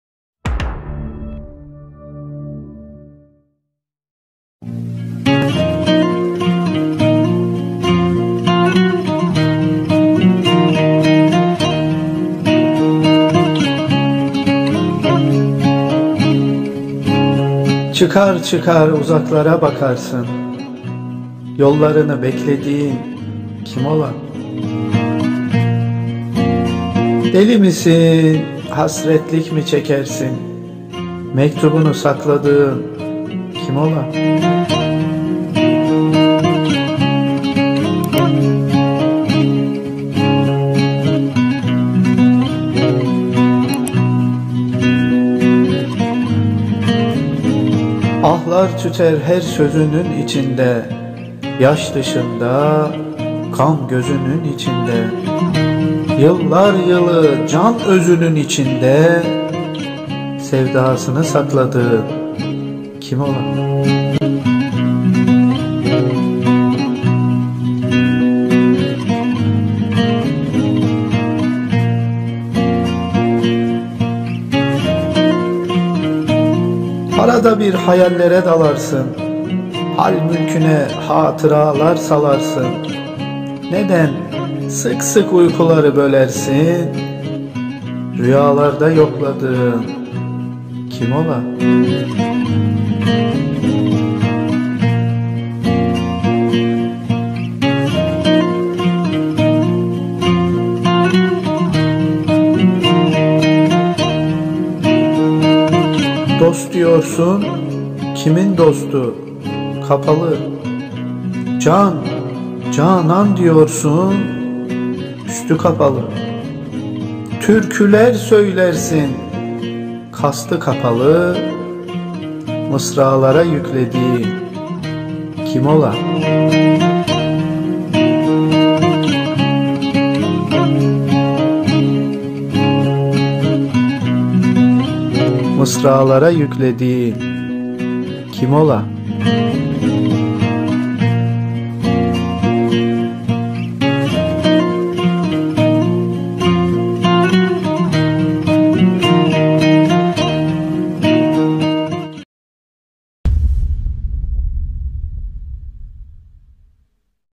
Abdurrahim Karakoç | Sordum amma Söylemez ki
Sordum amma Söylemez ki | Abdurrahim Karakoç

Sordum amma Söylemez ki

Çıkar çıkar uzaklara bakarsın
Yollarını beklediğin kim ola?
Deli misin, hasretlik mi çekersin
Mektubunu sakladığın kim ola?

Ah'lar tüter her sözünün içinde
Yaş dışında, gam gözünün içinde
Yıllar yılı can özünün içinde
Sevdasını sakladığın kim ola?

Arada bir hayallere dalarsın
Hâl mülküne hatıralar salarsın
Neden sık sık uykuları bölersin
Rüyalarda yokladığın kim ola?

Dost diyorsun, kimin dostu?
Kapalı Can, canân diyorsun üstü kapalı
Türküler söylersin kastı kapalı
Mısralara yüklediğin kim ola?